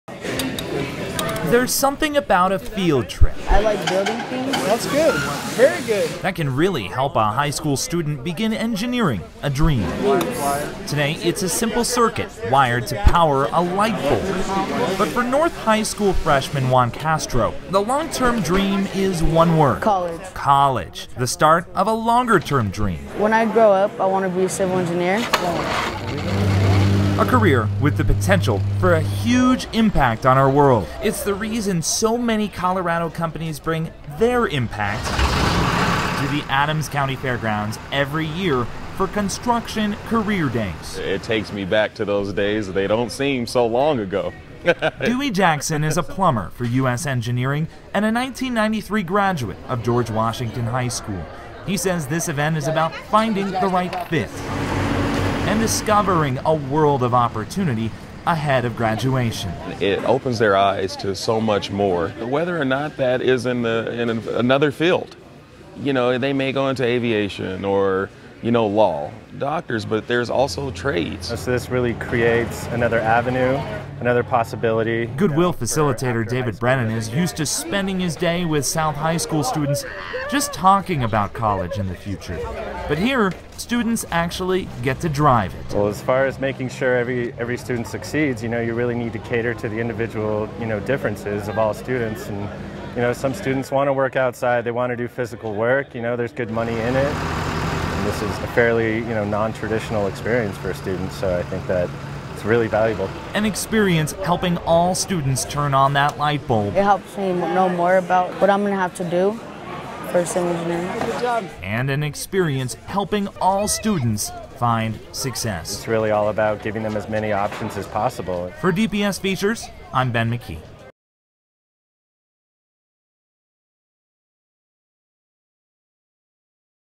Denver Students Dream Big At 2013 Colorado Construction Career Days
300 students from the Denver Public Schools had a hands-on opportunity to explore a collection of careers in construction, engineering, architecture, trade and mechanics at the 11th Annual Colorado Construction Career Days during the week of September 23-27.

The event, held at the Adams County Fairgrounds, could not have happened without the dozens of Colorado businesses who showcased a wide diversity of opportunities in trade and construction careers. Construction, Architectural, Engineering, and Energy industries provided interactive and informational booths to engage the hundreds of students visiting from across the state.